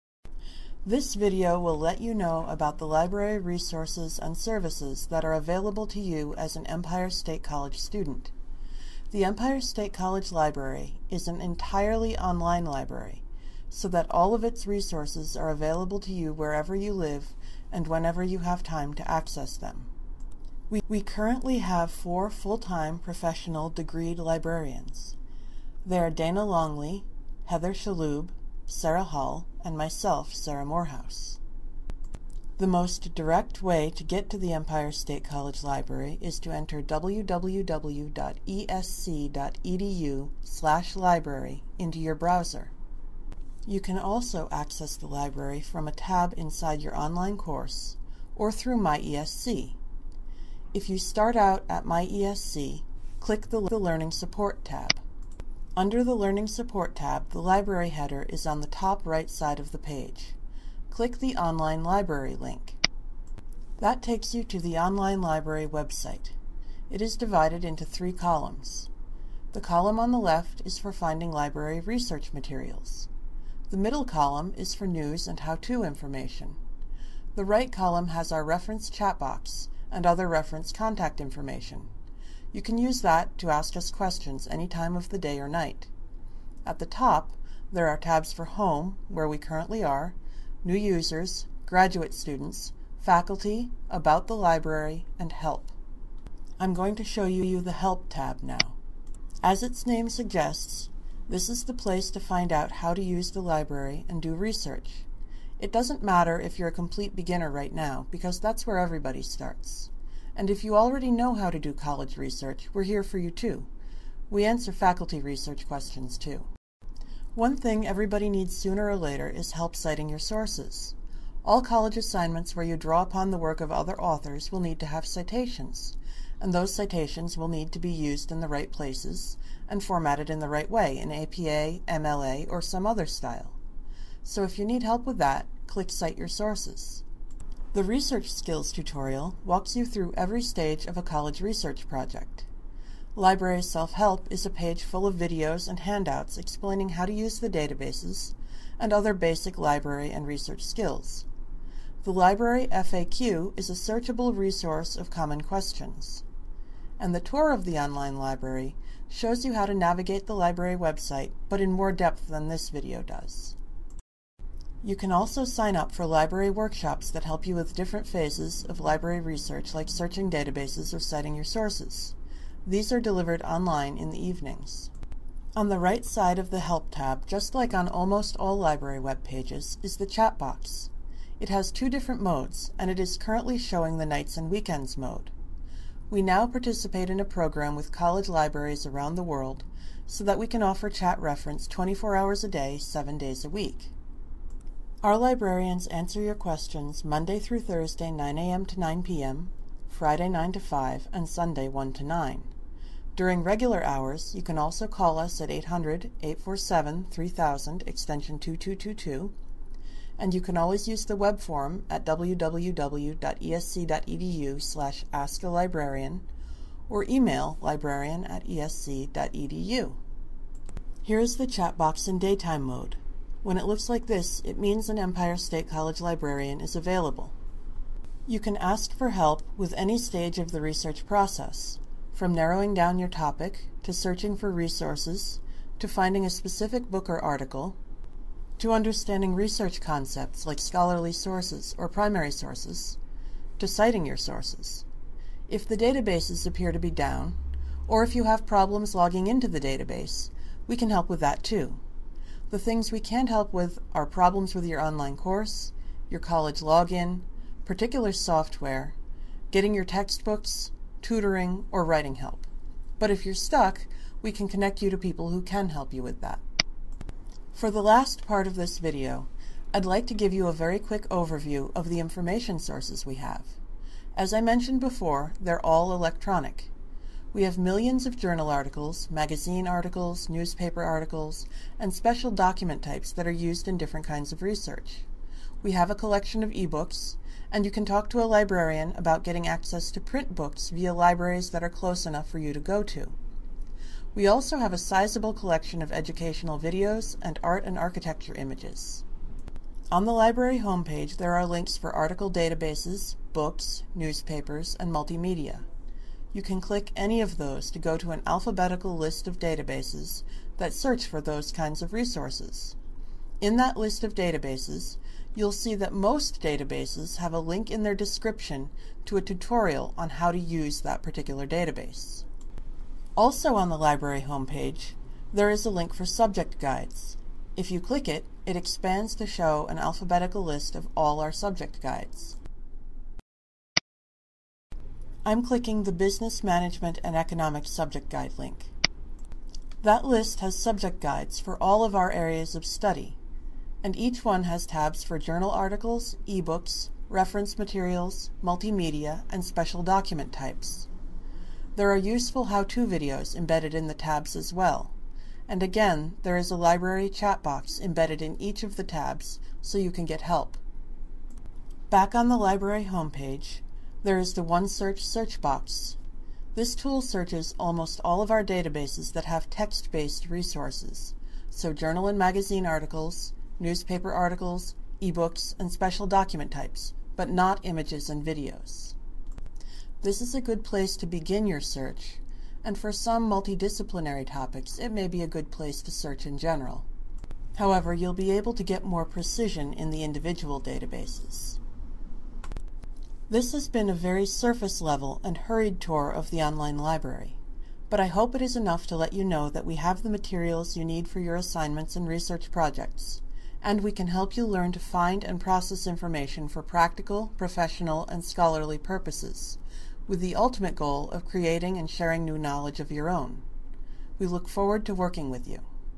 Intro the Empire State College Online Library
A quick video tour of how to get to the Library, how to get help, and how to find the information sources you need.